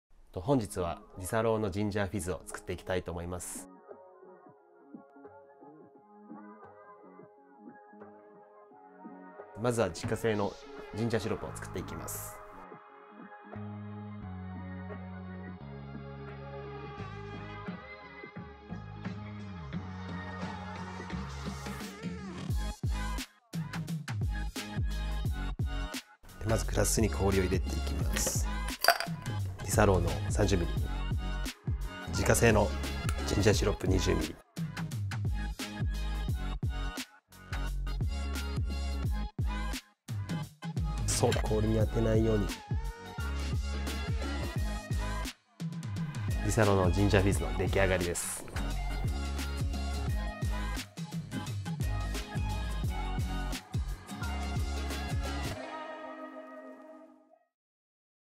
Make Your Own Disaronno #2 - Ginger Fizz Cocktail
Our favorite bartender and a big Disaronno fan, Keisuke Yamamoto, teaches us his special Ginger Fizz recepe using Disaronno.
待ちに待った春シーズンの到来に合わせ、今回はディサローノを使った爽やかなドリンク、ジンジャーフィズカクテルをご紹介します！
作り方を教えてくれるのは、レシピ動画でおなじみのジェレマイア Tokyoのヘッド・バーテンダー、山本圭介さん。

圭介さん一押しのスペシャル・レシピを伝授していただきました！

ディサローノの香りが引き立つ爽快なジンジャードリンクは、お花見やハウスパーティー、ピクニックなど春のイベントにもぴったり🌸
ぜひ今度の週末は、ディサローノと共にお過ごしください。

【DISARONNO Ginger Fizz ジンジャーフィズ・カクテル】

〈必要な材料〉
・ ディサローノ 30ml
・ 自家製ジンジャーシロップ 20ml *
・ ソーダ 70ml
・ レモンスライス 1枚
・ ローズマリー 適量

*自家製ジンジャーシロップの材料
・ レモン(スライス) 1/2個
・ 生姜 300g
・ シナモン 1/2個
・ 鷹の爪 1個
・ 砂糖 300g
・ 水 300cc
・ クローブ 5粒

春にぴったりな爽やかな一杯。
人気フレアバー、ジェレマイア Tokyoでお楽しみいただけます。

皆さんもぜひ足を運んでみてください🍸

圭介さんのオフィシャルSNSページも要チェックです！